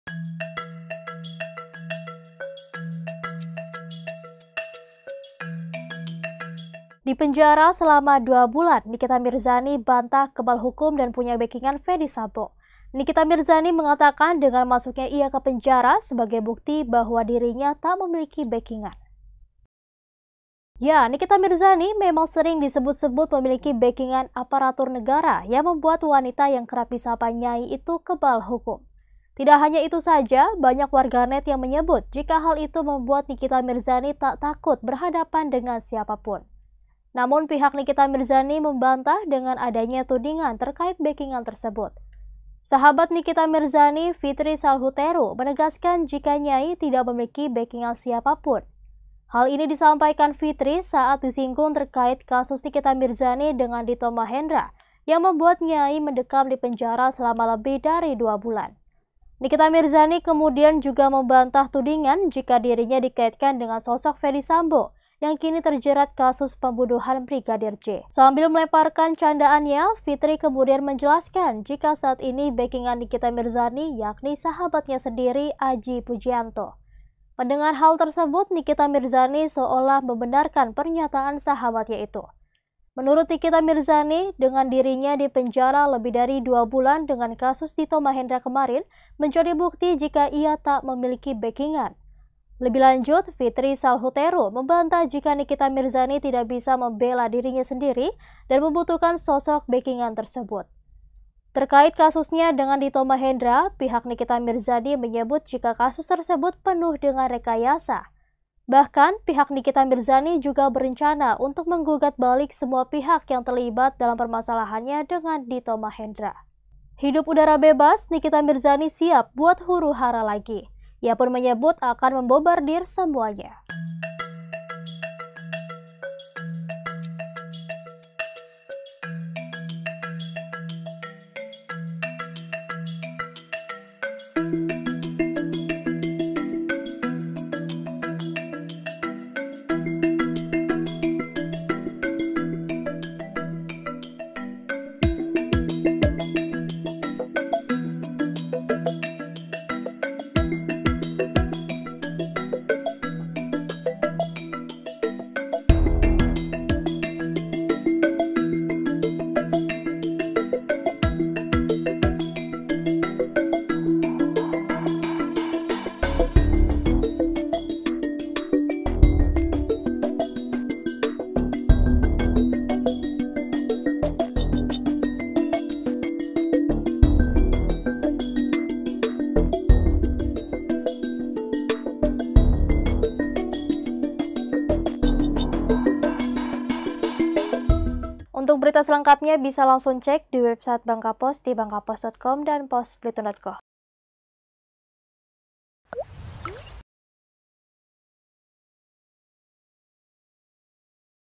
Nikita Mirzani Bantah Kebal Hukum dan Punya Bekingan Ferdy Sambo
Dipenjara dua bulan, Nikita Mirzani bantah kebal hukum dan punya bekingan Ferdy Sambo.